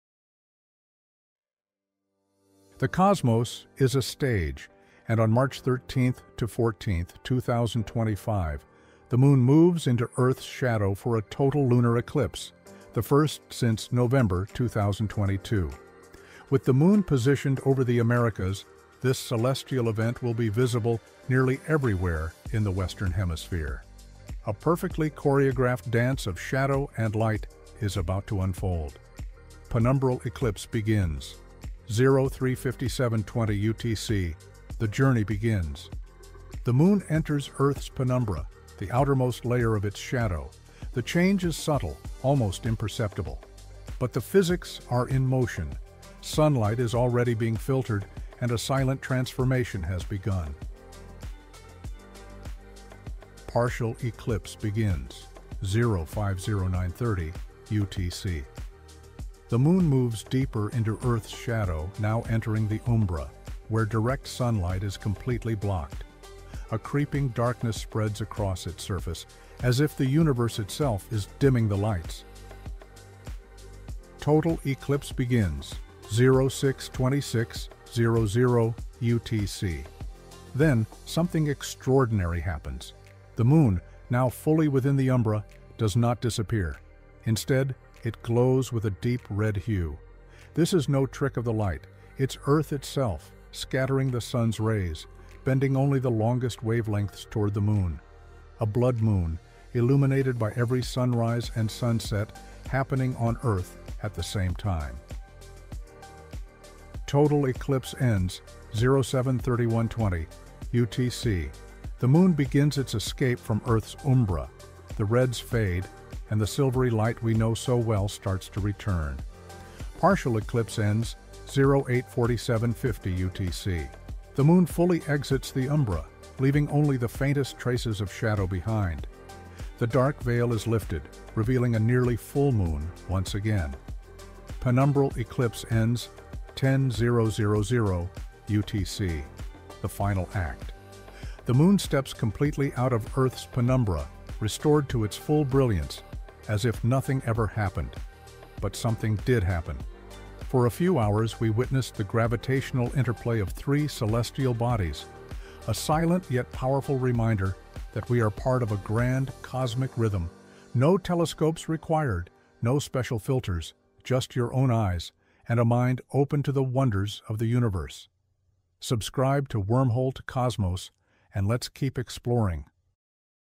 March 13–14, 2025: Total Lunar Eclipse and Blood Moon Over the Americas
🌕✨ Experience the Total Lunar Eclipse on March 13–14, 2025! ✨🌕

On the night of March 13–14, 2025, the Moon will pass into Earth's shadow, creating a total lunar eclipse—the first since November 2022. This celestial event will be visible across the Americas, offering a spectacular view of the Moon turning a deep red hue, commonly referred to as a "Blood Moon."
SCIENCE.NASA.GOV

Eclipse Timeline:

Penumbral Eclipse Begins: 03:57 UTC
Partial Eclipse Begins: 05:09 UTC
Total Eclipse (Blood Moon): 06:26 – 07:31 UTC
Partial Eclipse Ends: 08:48 UTC
Penumbral Eclipse Ends: 10:00 UTC
This total lunar eclipse will be visible in its entirety across North and South America, providing a prime viewing opportunity for observers in these regions.
SPACE.COM

Unlike solar eclipses, lunar eclipses are safe to view with the naked eye, binoculars, or a telescope. No special equipment is required to enjoy this astronomical phenomenon.
SCIENCE.NASA.GOV

Join us as we explore the science behind this captivating event, delving into the mechanics of lunar eclipses and the reasons behind the Moon's dramatic color change during totality. Don't miss this opportunity to witness one of nature's most awe-inspiring spectacles.

Credit : NASA's Scientific Visualization Studio